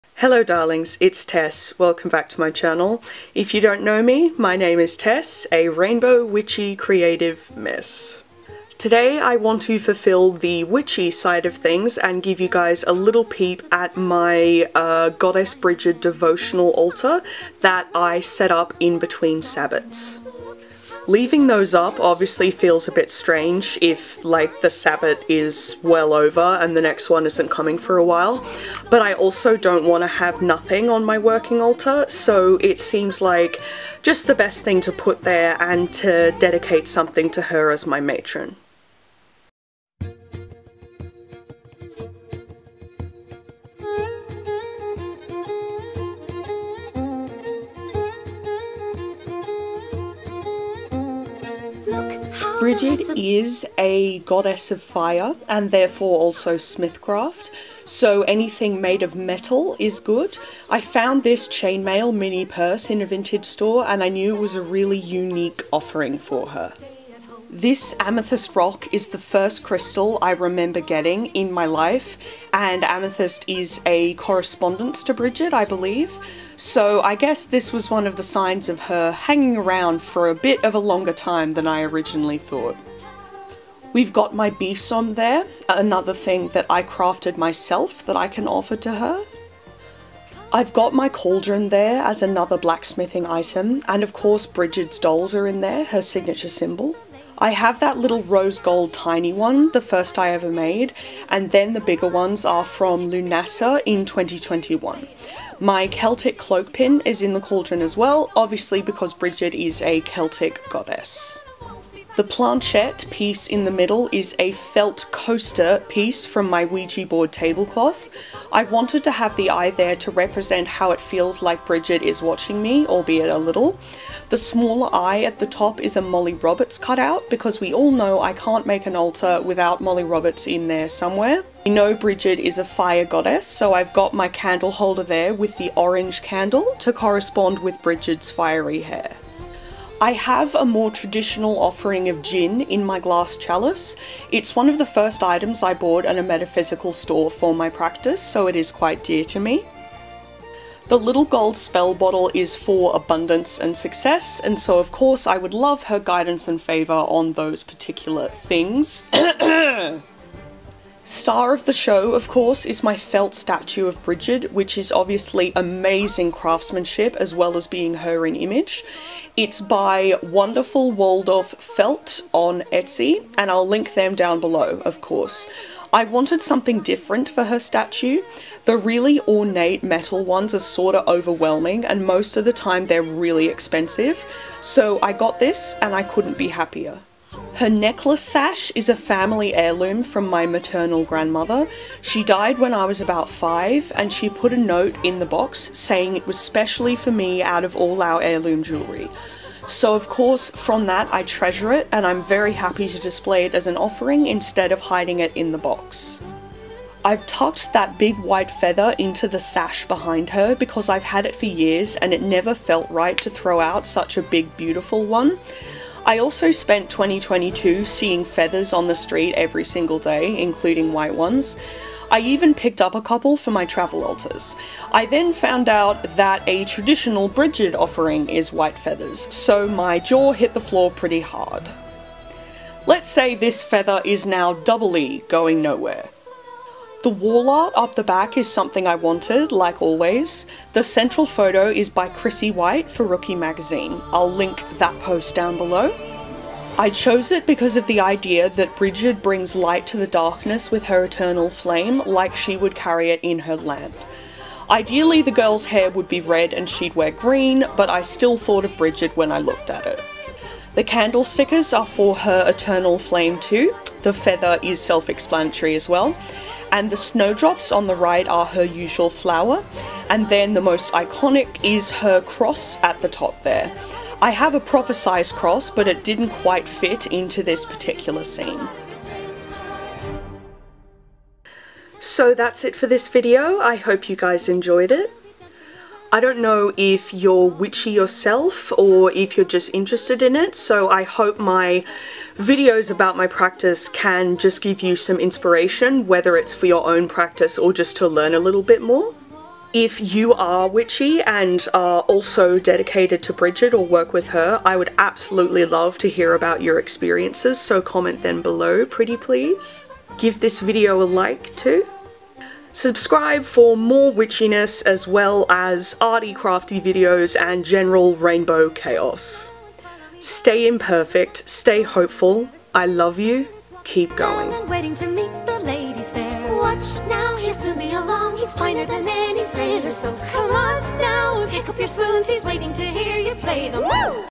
Brigid Devotional Altar
Celtic goddess Brigid rules over fire, smithcraft and creativity. She is my matron for the latter reason and I wanted to dedicate a specific space to her. I centred it around my amazing statue of her from Wonderful Waldorf Felt (on Etsy) and added other corresponding offerings to her, like my Brigid's dolls and a chalice of gin. Brigid, I honour you. I would love to have a bigger spot for her one day, maybe another statue, but for now I love my little space here and I pay tribute to it as often as I can. I love you, keep going!